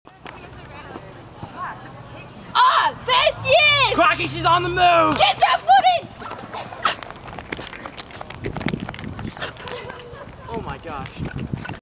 In the Australian Outback pt.2
Landon and Tiler follow the life of Staph Infection